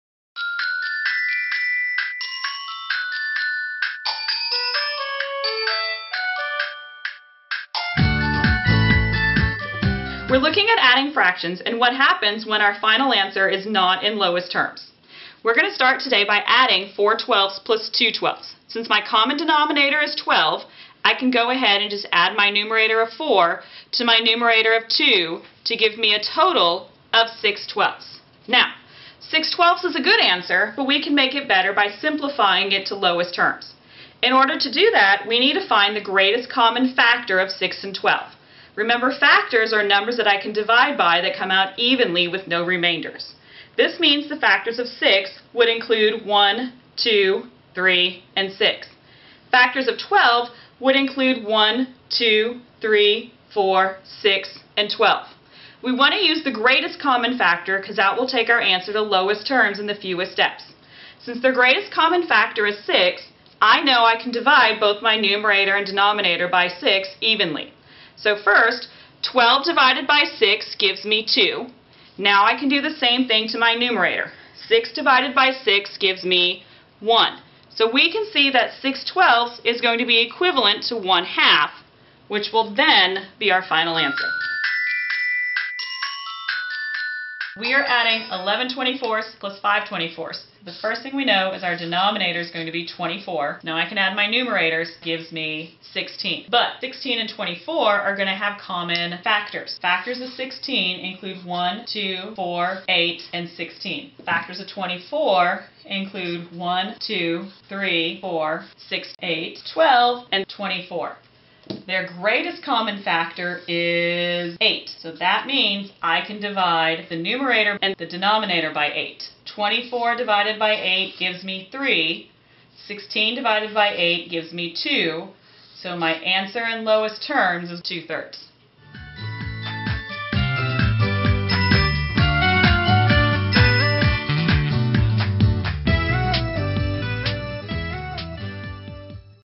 How to Simplify the Sum | Adding Fractions and Mixed Numbers
Learn what to do when your sum is not lowest terms and how to simplify it.